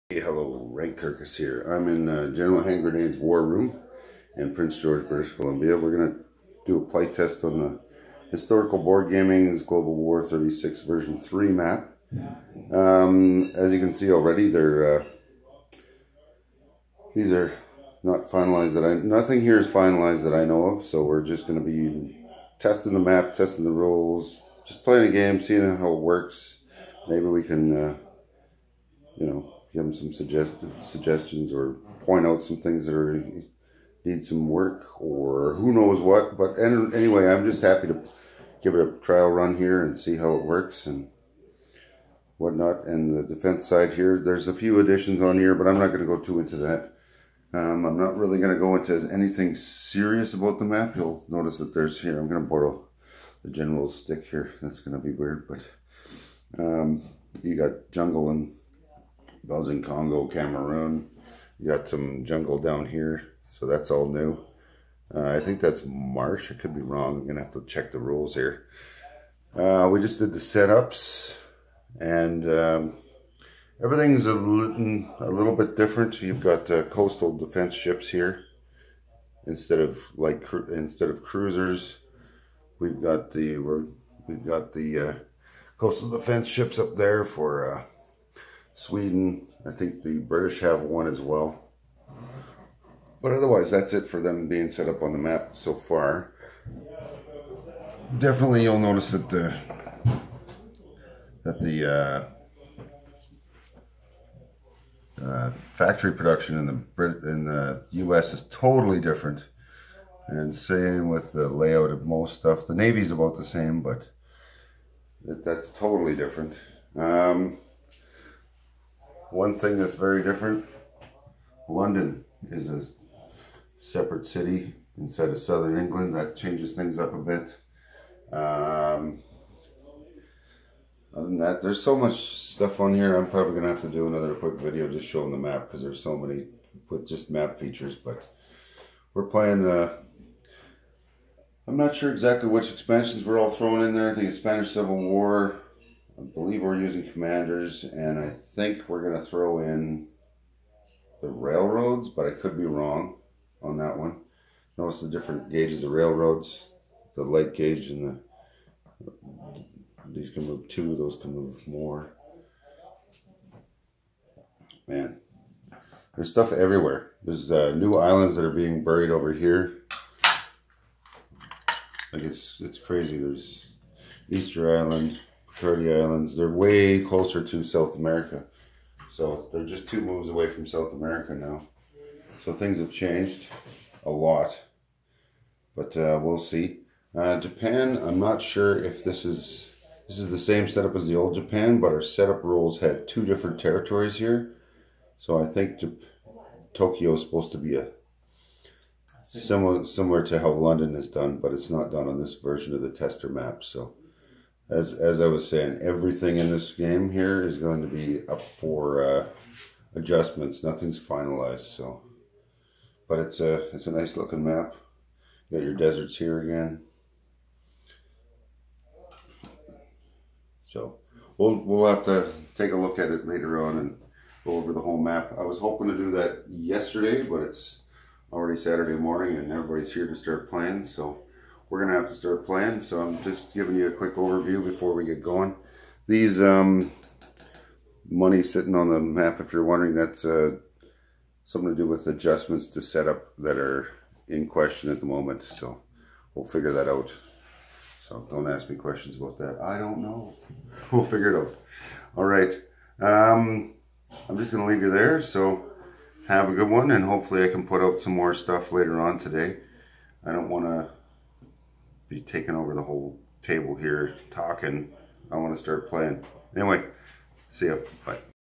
Tanks on the Tundra intro
Overview of map and setup. Prior to game playtesting Global war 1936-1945 version 3 made by Historical Board Gaming. com